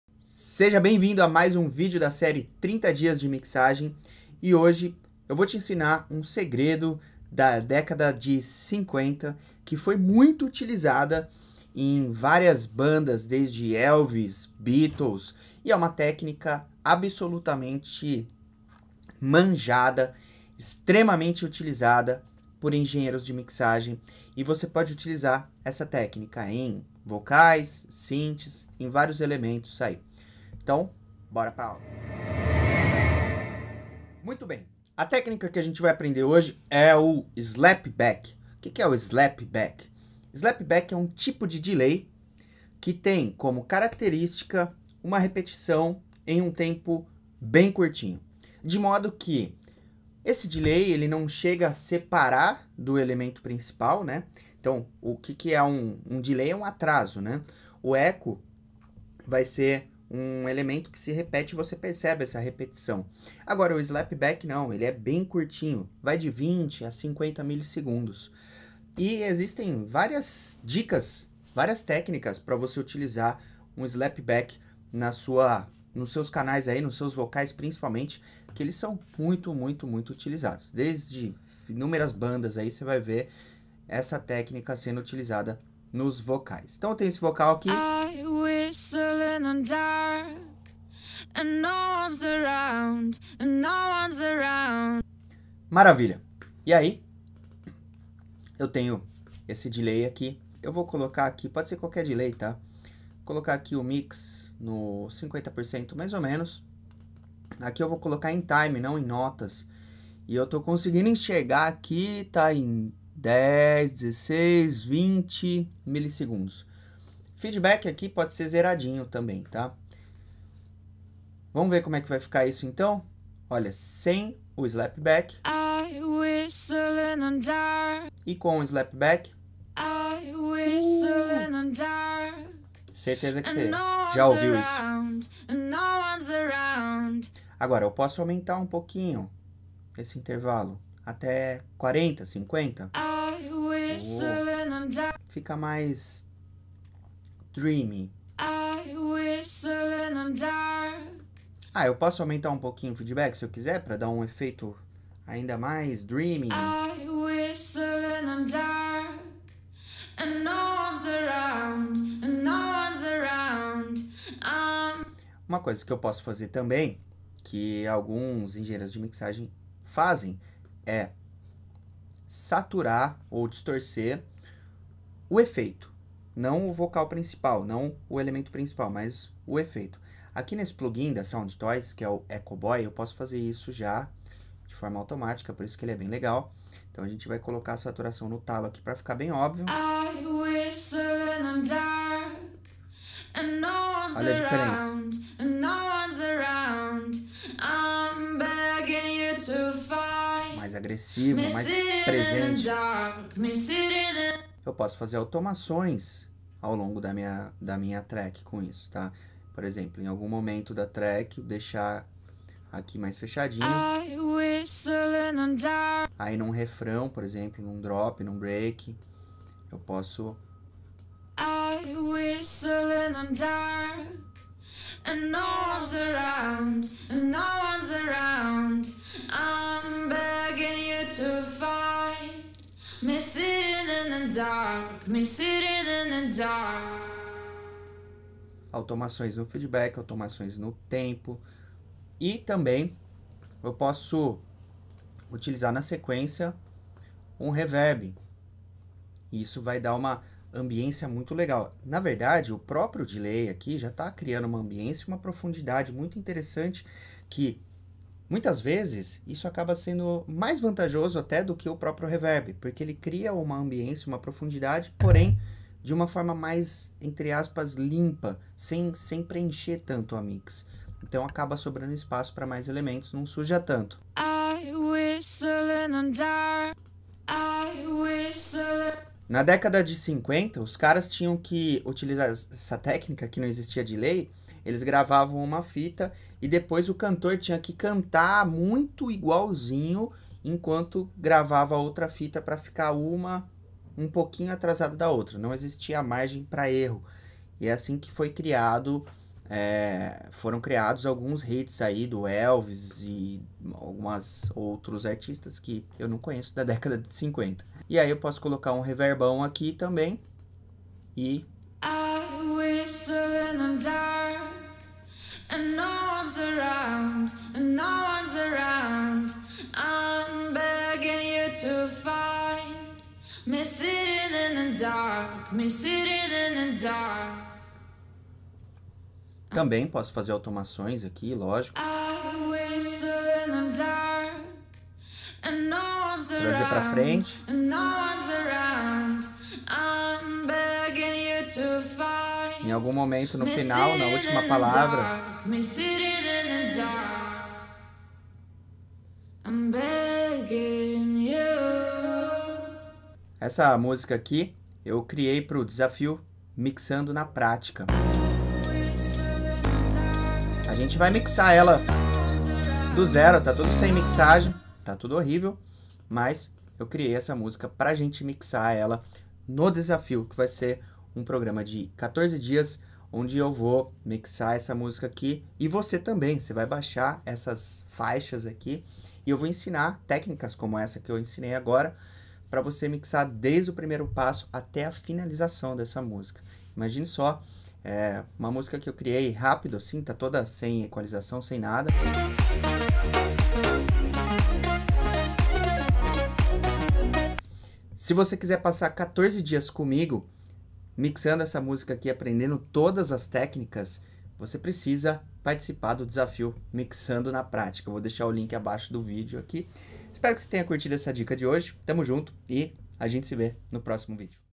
Dica #3 - Copie Este Truque dos Engenheiros de Mix
A técnica deste vídeo é muito conhecida há mais de 70 anos.
Ela esteve em gravações do Elvis e Beatles.

Mas, agora, você não precisa mais ter tanto trabalho como eles tiveram porque temos o privilégio de viver na era dos plugins!

Nesse vídeo, você vai conhecer o Slap Back Delay!

💎  DICAS QUENTES NO INSTAGRAM: @eduardojuliato💎